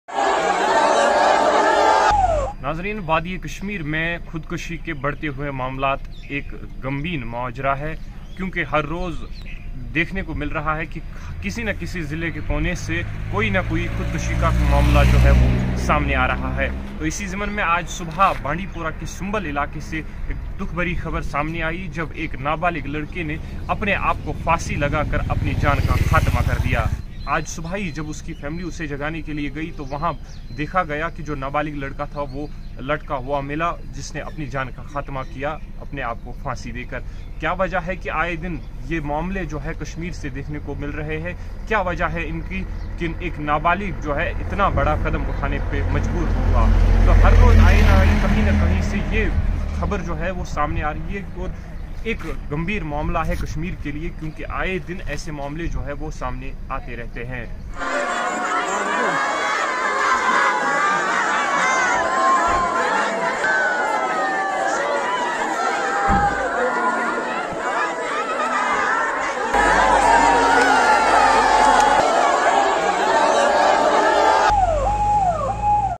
Teenager hangs self to de_ath in Sumbal Village
Teenager hangs self to death in Sumbal Village

Bandipora: A teenage boy allegedly committed suicide by hanging himself at his home in Bathipora village of Sumbal in north Kashmir's Bandipora district, officials said here on Tuesday.

An official told the news agency—Kashmir News Observer (KNO), that early morning the boy was found hanging at his residence by his family members.

"It was not immediately known as to why he took this extreme step," he said.

Meanwhile, police have taken cognizance of this incident and further investigation has been initiated—(KNO)